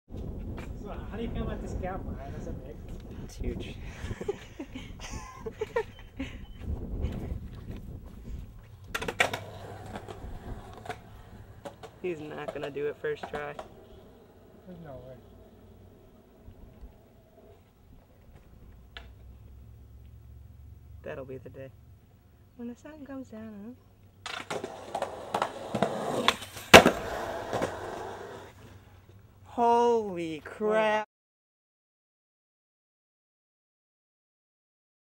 Strawberry Burgers Skateboarding Promo
Promo for the colabo video between strawberry skate team and the infamous BURGER BOARDS